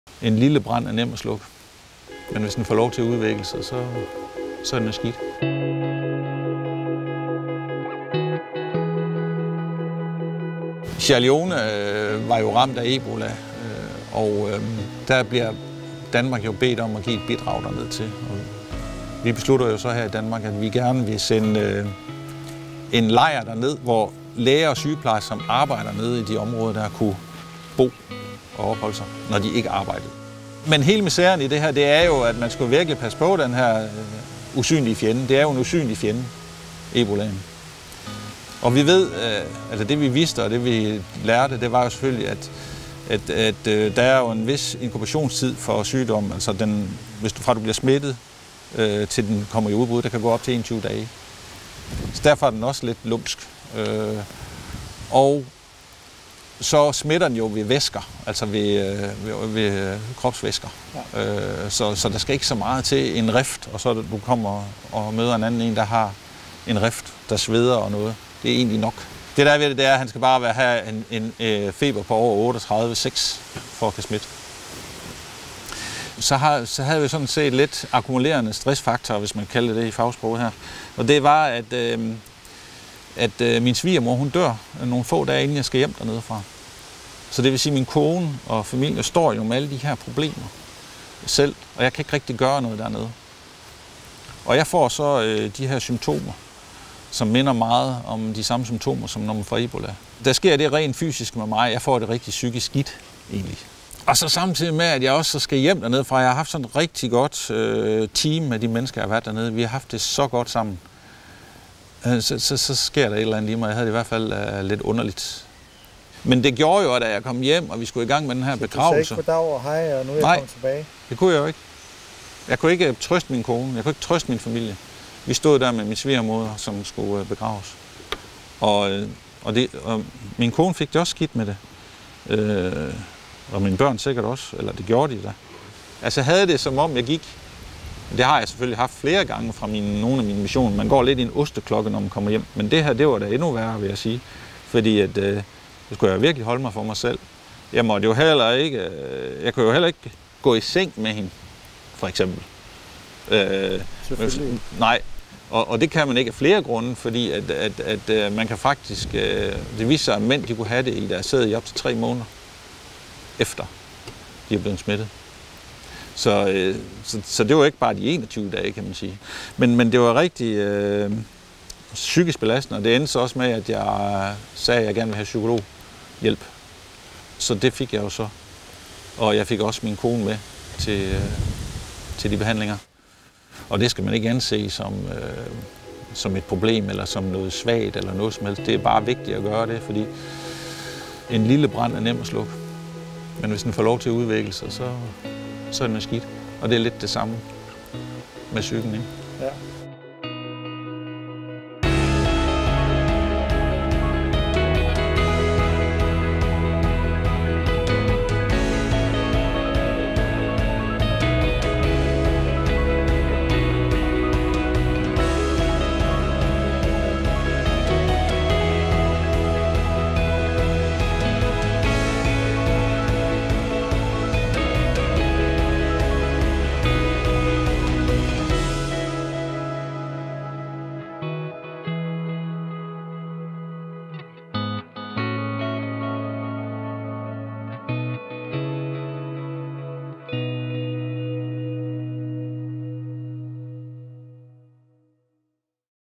Ebola osteklokken
”Jeg kunne ikke trøste min kone. Jeg kunne ikke trøste min familie. Vi stod der med min svigermor, som skulle begraves, og min kone og mine børn fik det skidt med det,” fortæller Jørgen, der med Beredskabsstyrelsen er udsendt til Sierra Leone i 2014 og 2015. Her er verdenshistoriens største udbrud af den smitsomme sygdom i gang med at slå over 10.000 mennesker ihjel.

Danmark sender beredskabsfolk, læger og sygeplejersker ned til byen Port Loko, hvor de træner lokale og internationale sundhedsmedarbejdere i at håndtere sygdommen. På de ni måneder Danmark er dernede, bliver mere end 5.000 uddannet.

Den usynlige fjende
Beredskabsfolkene lærer hurtigt at omgås hinanden og lokalbefolkningen med stor varsomhed, for sygdommen er en lumsk og synlig fjende, der smitter gennem kropsvæsker. ”Der skal ikke så meget til. En rift og du møder en anden, der har en rift, der sveder – det er nok. Han skal bare have en feber på over 38,6 for at kunne smitte,” siger Jørgen.

Han får selv symptomer, der minder om ebola. Det sker samtidig med, at han står foran at skulle hjem til sin familie, hvor hans kones mor netop er død.